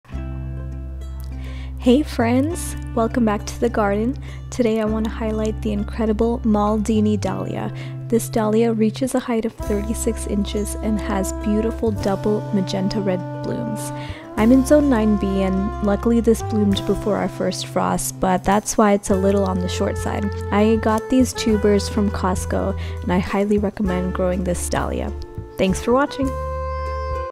Growing Maldini Dahlia in Zone 9B
My first time growing the Maldini dahlia in my garden. This is actually the first dahlia I have successfully grown from tuber, bought through Longfield Garden in Costco. I planted the tuber in November and got a small bloom before frost! Dahlias should overwinter in my zone so I look forward to more blooms this year.
Please let me know your favorite dahlia varieties in the comments below!